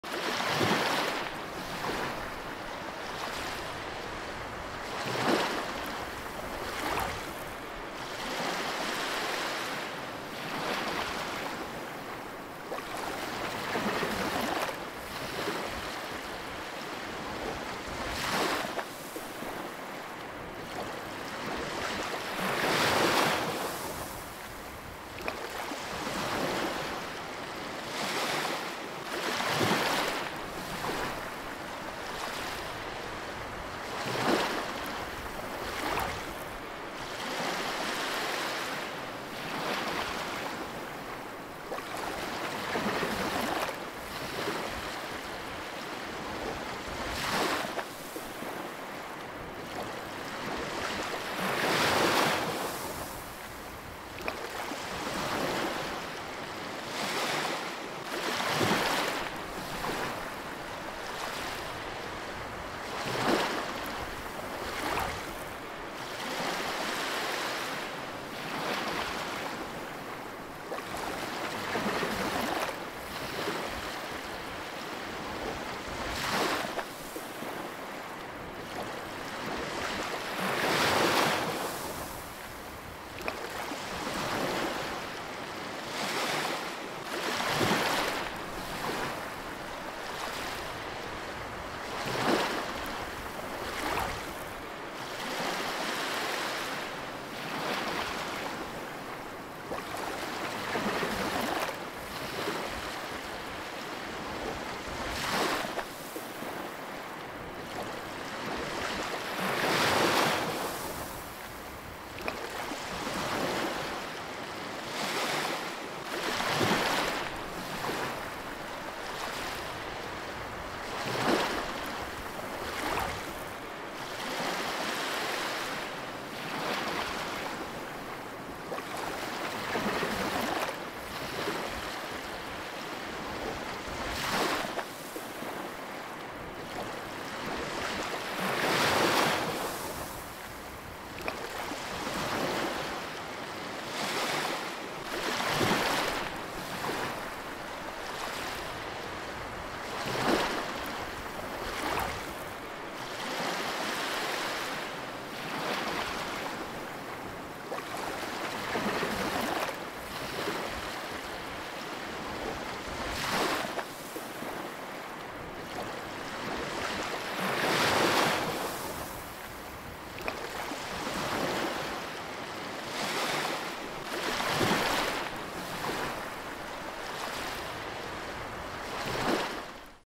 CALMING SOUND - Close Sea Waves
A 3-minute close sea waves sound to relax your mind.

Close sea waves, with their gentle and persistent rhythm, weave a tranquil tapestry of serenity along the shore. The soothing sounds of waves breaking near create a lullaby, inviting a sense of calm and introspection. Each wave's embrace whispers a story of the sea, fostering a meditative atmosphere that washes away stress and carries the mind into a state of peaceful contemplation.